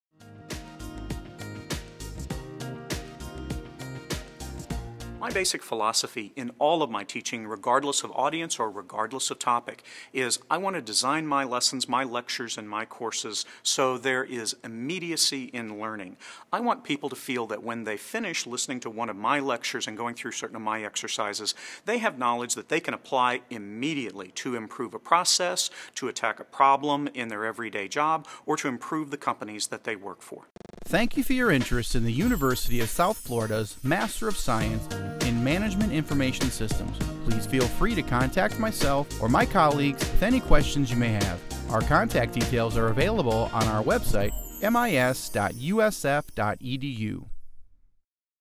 How are the Courses Optimally Designed Within the Program?
For more information on USF's nationally recognized and highly ranked program,

Part of the Information Systems and Decision Sciences Department, Ron Satterfield, PhD, is an instructor who teaches business and economics courses in statistics as well as courses in operations management, service operations management, mathematical modeling and lean management. He has taught courses in the "business of medicine," delivering business education to physicians and other healthcare professionals.